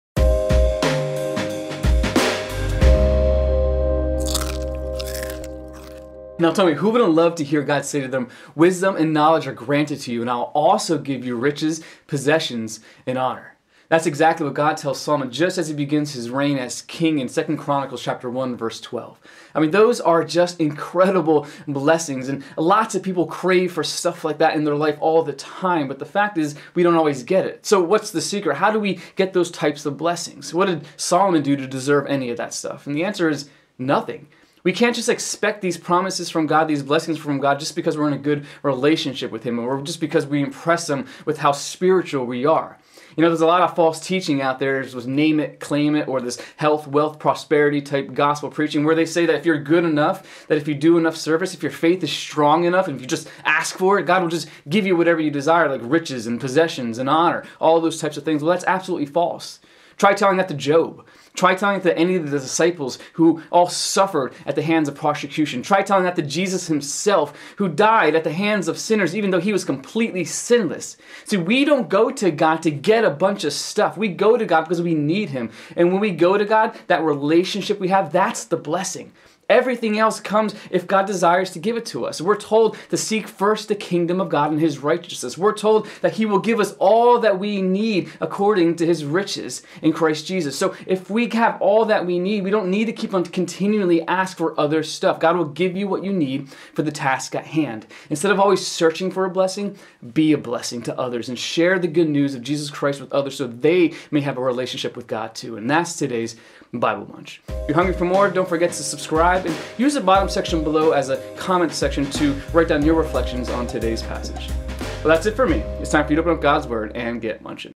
Name it, Claim it Blessings? | 2 Chronicles 1:12 Bible Devotional | Christian Vlogger
What do we need to do to get blessings from God? Are blessings like health, wealth, and prosperity promised to all Christians? If our faith is strong enough, does the Bible tell us to just name it, claim it to receive what we want? Watch this Bible devotional taken out of 2 Chronicles 1:12, to get the answers to those questions.

2 Chronicles 1:12 - Wisdom and knowledge are granted to you. I will also give you riches, possessions, and honor, such as none of the kings had who were before you, and none after you shall have the like.

Next vlog ► This Thursday!